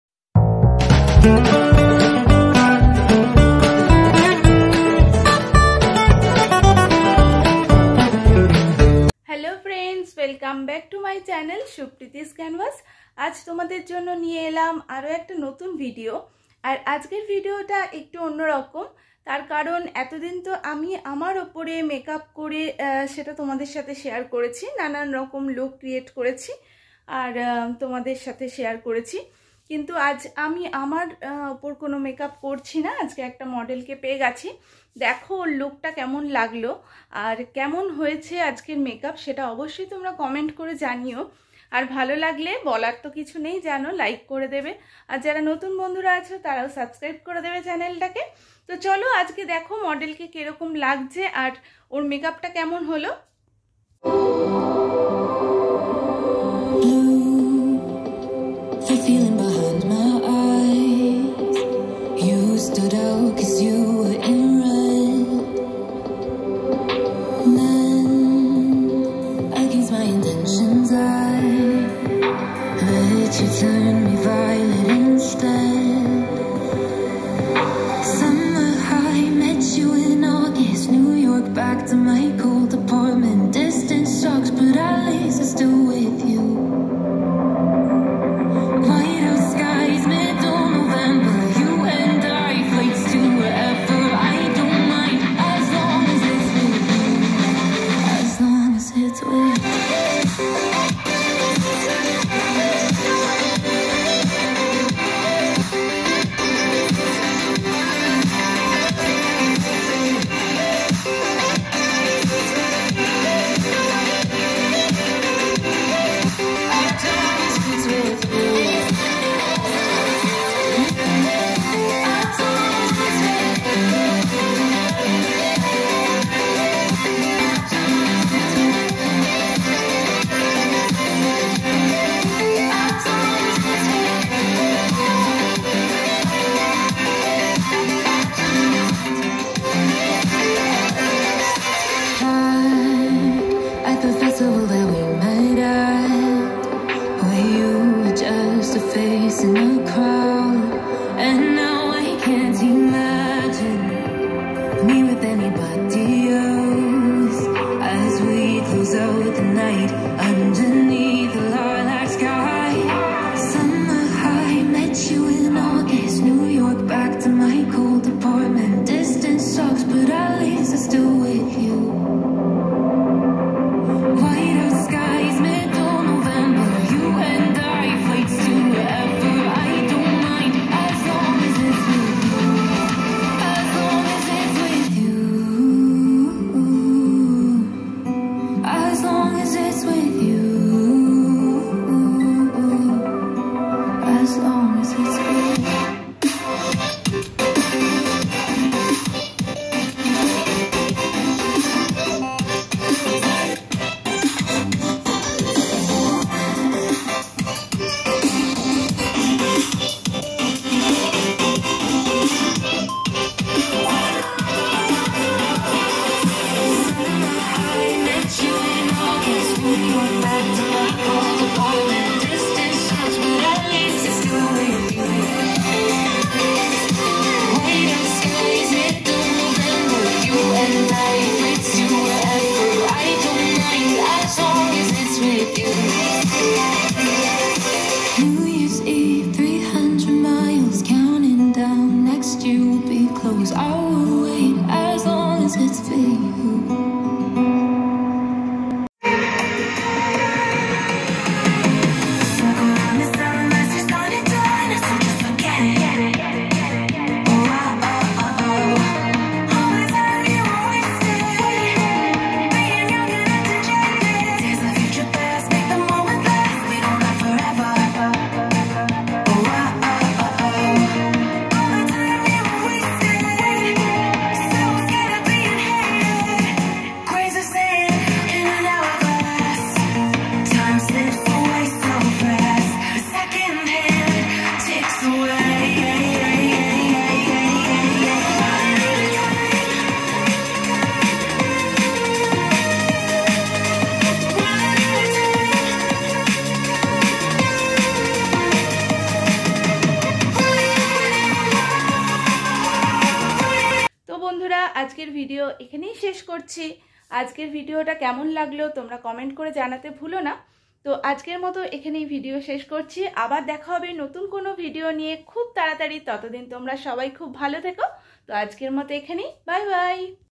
Create a golden makeover for occasion/ glamorous gorgeous makeup look
Today I create a golden makeup look which give us gorgeous and attractive look.It is very easy to create so all friends please watch full video and like or new friends please subscribe this channel .
makeup products
eyeshadow pallet- makeup revolution
foundation- PAC
concealer- PAC
lipstick pallet-missclear